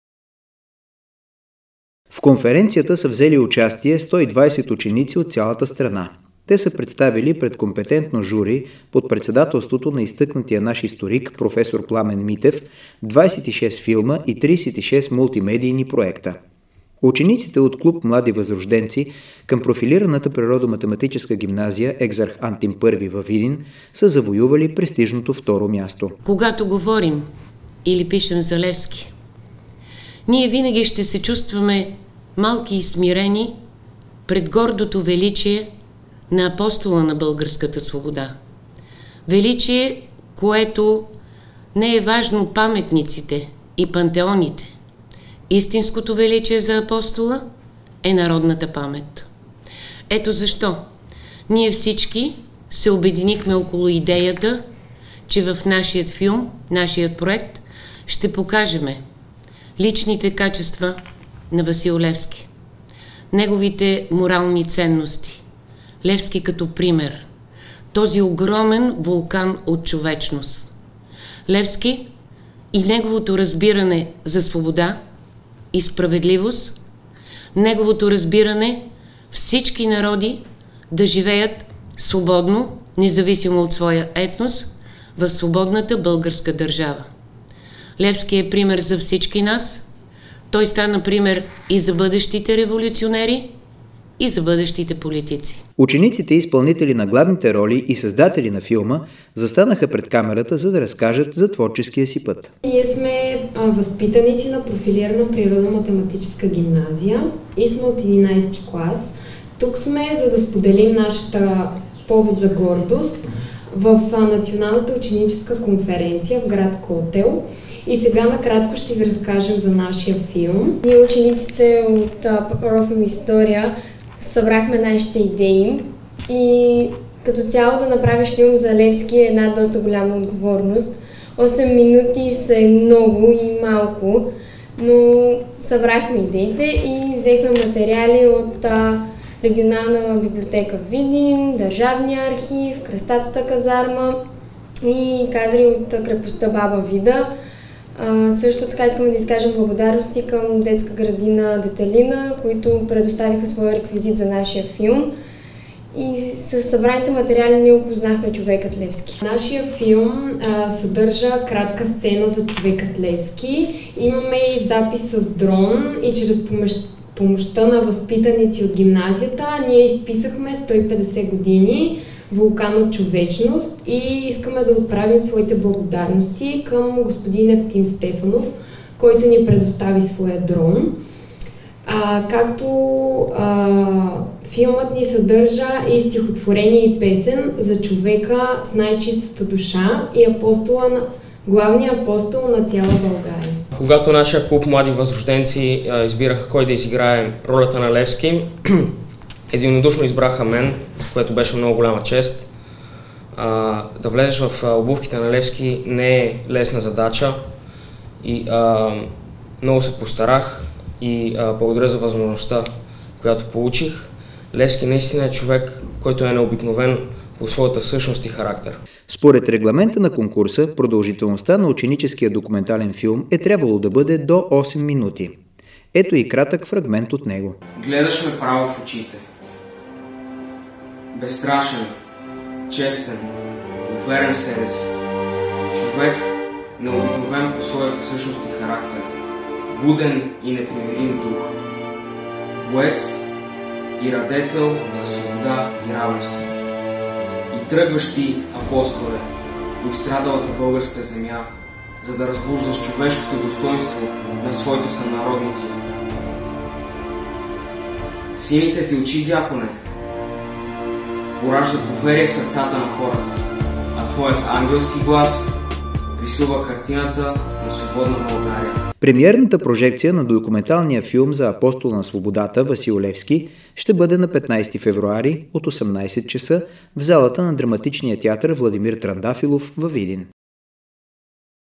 Филм за Васил Левски на видински ученици от ППМГ спечели награда от национален конкурс
Документалният филм спечели второ място на Национална ученическа конференция „Васил Левски – вулкан от човечност“, посветена на 150 години от гибелта на Апостола, която се проведе в град Котел.